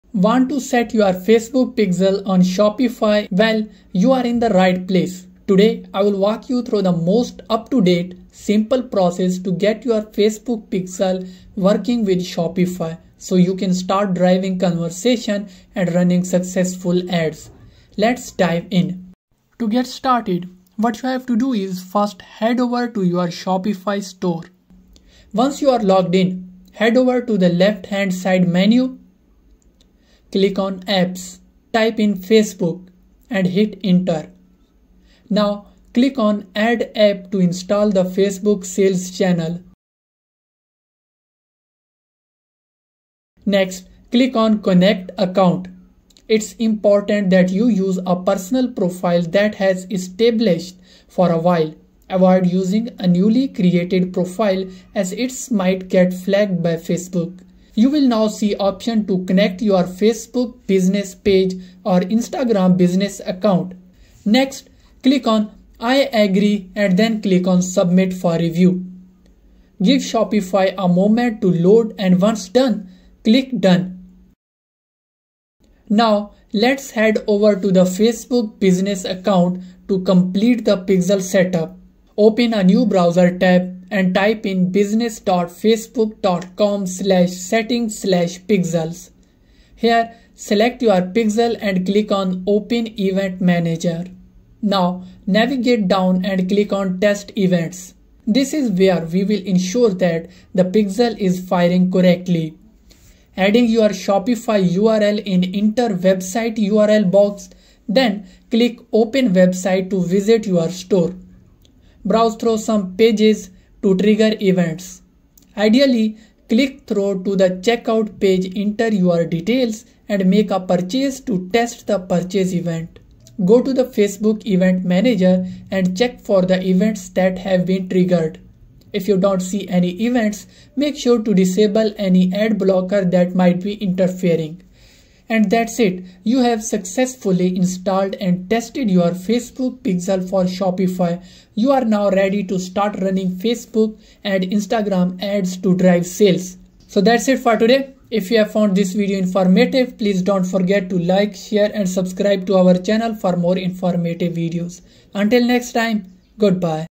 How to Create Pixel in Facebook Ads for Shopify
Want to track your Shopify sales on Facebook? Learn how to easily set up the Facebook Pixel for your Shopify store. Our step-by-step guide will show you how to create and install the pixel for better ad targeting.